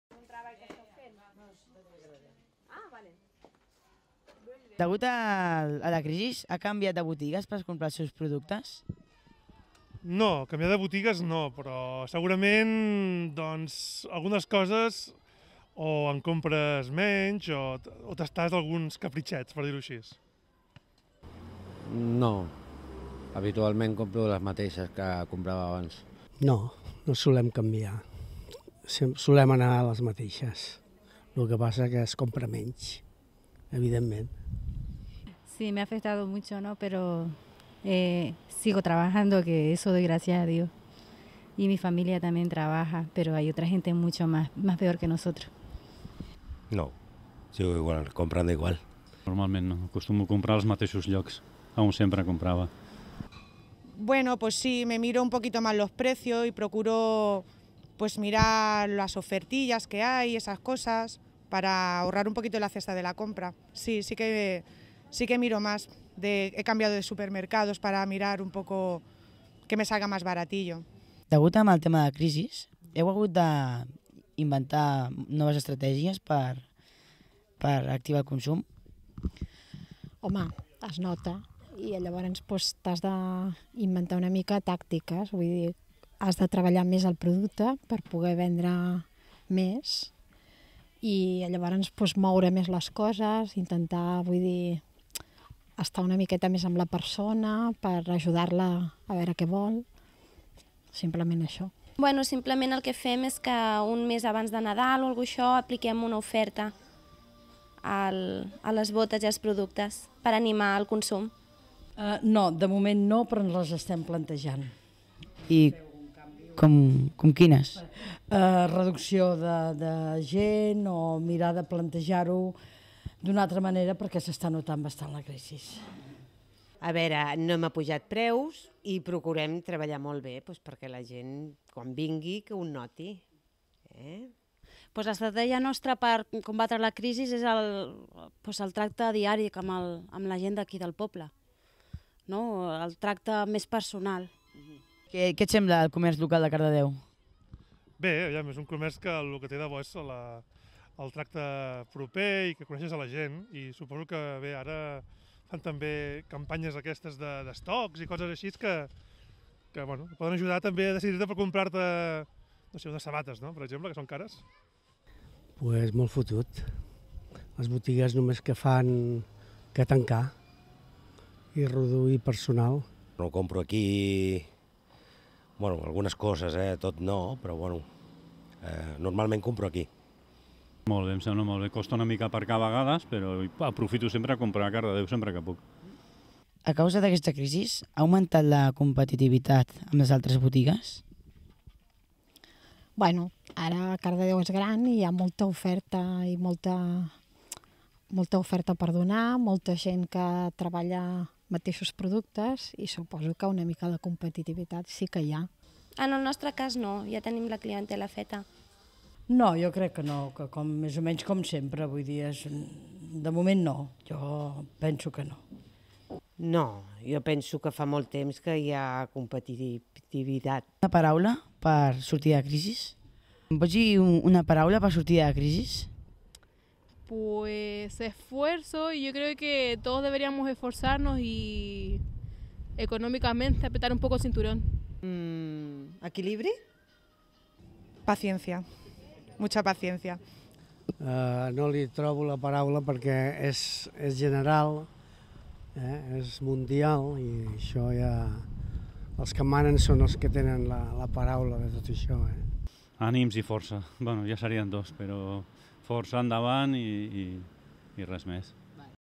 Imaginació contra la crisi
Sobre la crisi hi ha moltes opinions i s'ha dit de tot, però des de TVCardedeu hem volgut saber què n'opinen els botigers i si fan estratègies per combatre-la. Igualment també hem preguntat en els cardedeuencs com veuen la crisi i si compren en el comerç local.